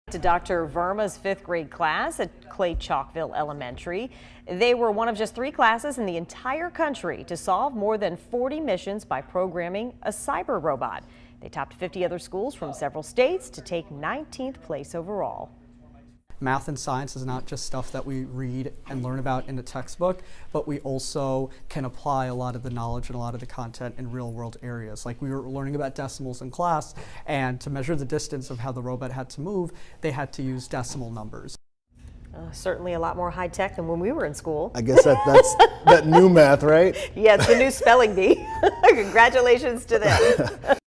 5th graders from Chalkville Elementary earn national honors in robotics competition
5th graders from Chalkville Elementary earn national honors in robotics competition